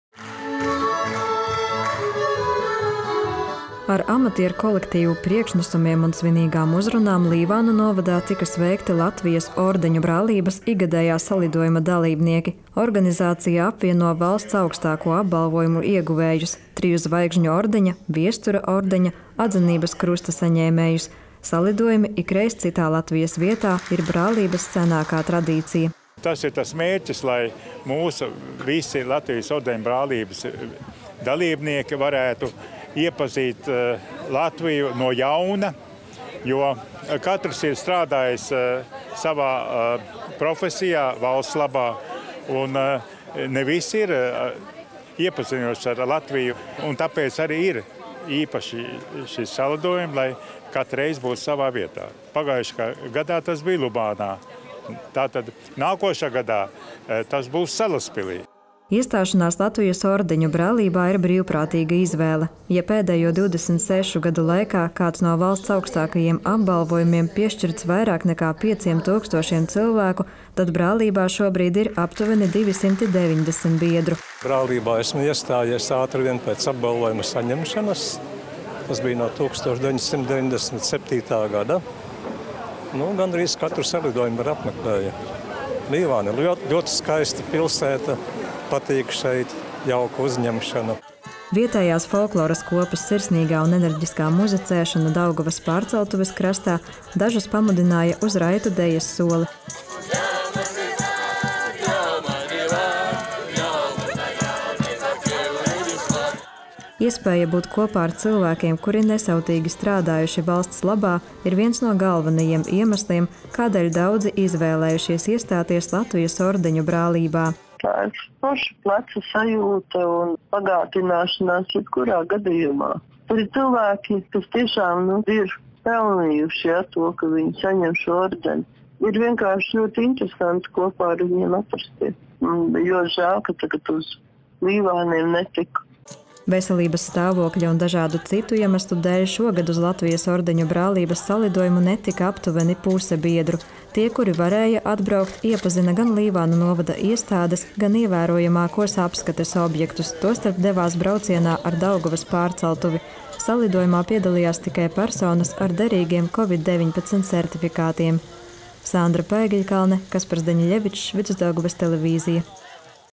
ReTV: Valsts augstāko apbalvojumu ieguvēji pulcējas 23.salidojumā
23.salidojumu aizvadījusi Latvijas Ordeņu brālība. Organizācija, kurā apvienoti valsts augstāko apbalvojumu ieguvēji, ik gadu rīko biedru salidojumu kādā no Latvijas vietām, un šoreiz pasākums norisinājās Līvānu novadā.

 Ar amatierkolektīvu priekšnesumiem un svinīgām uzrunām Līvānu novadā tika sveikti Latvijas Ordeņu brālības ikgadējā salidojuma dalībnieki. Organizācija apvieno Valsts augstāko apbalvojumu ieguvējus - Triju Zvaigžņu ordeņa, Viestura ordeņa, Atzinības krusta saņēmējus. Salidojumi ikreiz citā Latvijas vietā ir brālības senākā tradīcija. “Tas ir tas mērķis, lai mūsu visi Latvijas Ordeņu brālības dalībnieki varētu iepazīt Latviju no jauna, jo katrs ir strādājis savā profesijā valsts labā, ne visi ir iepazinuši Latviju, tāpēc arī ir īpaši šie salidojumi, lai katrreiz būtu savā vietā. Pagājušogad bija Lubānā, nākamgad tas būs Salaspilī,” Juris Griķis, Latvijas Ordeņu brālības domes un valdes priekšsēdētājs.

Iestāšanās Latvijas Ordeņu brālībā ir brīvprātīga izvēle. Ja pēdējo 26 gadu laikā kāds no valsts augstākajiem apbalvojumiem piešķirts vairāk nekā 5000 cilvēku, tad brālībā šobrīd ir aptuveni 290 biedru. “Brālībā esmu iestājies ātri vien pēc apbalvojuma saņemšanas, tas bija 1997.gadā, gandrīz katru salidojumu arī apmeklēju. Līvāni ir ļoti skaista pilsēta, patīk šeit, jauka uzņemšana,” saka Ivars Liepiņš, Triju Zvaigžņu ordeņa Goda zīmes saņēmējs.

Vietējās folkloras kopas sirsnīgā, enerģiskā muzicēšana Daugavas pārceltuves krastā dažus pamudināja uz raitu dejas soli. Iespēja būt kopā ar cilvēkiem, kuri nesavtīgi strādājuši valsts labā, ir viens no galvenajiem iemesliem, kādēļ daudzi izvēlējušies iestāties Latvijas Ordeņu brālībā. “Tā ir forša pleca sajūta un bagātināšanās jebkurā gadījumā. Tur ir cilvēki, kas tiešām ir pelnījuši to, ka viņi saņem šo ordeni. Ir vienkārši ļoti interesanti kopā ar viņiem atrasties. Man bija ļoti žēl, ka tagad uz Līvāniem netiku,” pauž Virdžīnija Lejiņa, režisore, Atzinības krusta virsniece.

Veselības stāvokļa un dažādu citu iemeslu dēļ šogad uz Latvijas Ordeņu brālības salidojumu netika aptuveni puse biedru. Tie, kuri varēja atbraukt, iepazina gan Līvānu novada iestādes, gan ievērojamākos apskates objektus, tostarp devās braucienā ar Daugavas pārceltuvi. Salidojumā piedalījās tikai personas ar derīgiem Covid-19 sertifikātiem.